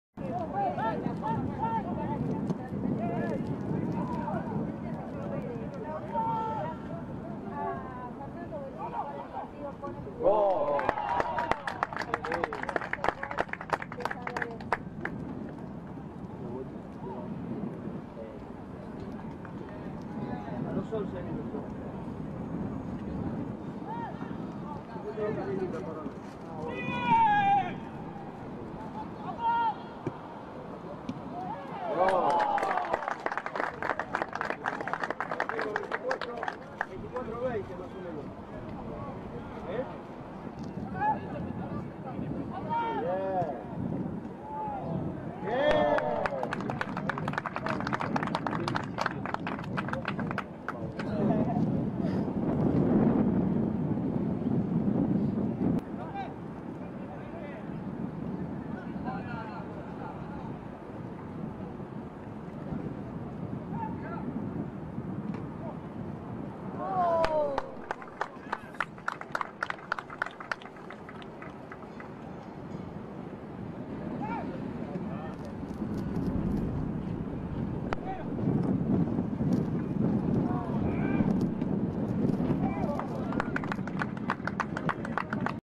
Resumen: San Lorenzo 5-0 Atl. Tucumán | Reserva - Fecha 7
Con goles del "Mágico" González (x2), Alexander Díaz (x2) y Gonzalo Berterame, SanLorenzo derrotó 5-0 a Atl. Tucumán en Ciudad Deportiva, por la séptima fecha de la Superliga. Fernando Belluschi completó 45' minutos. ¡Vamos los pibes!

Código Azulgrana, los códigos de esta pasión. 🔵🔴